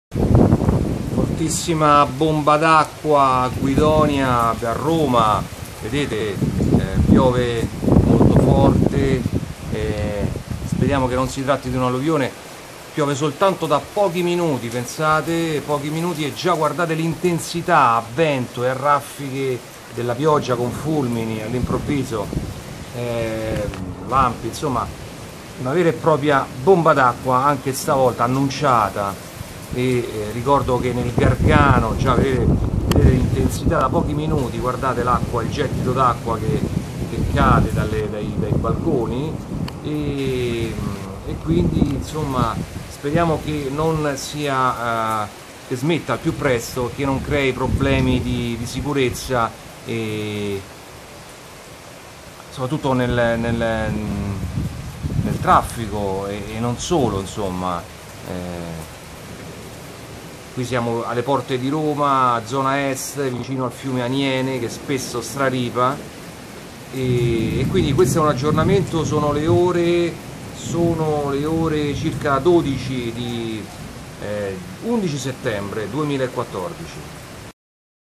Bomba d'Acqua a Roma Guidonia - Pericolo Alluvione : Video
Video di una bomba d'acqua a Guidonia, Roma: pericolo di alluvione nella capitale e zone limitrofe.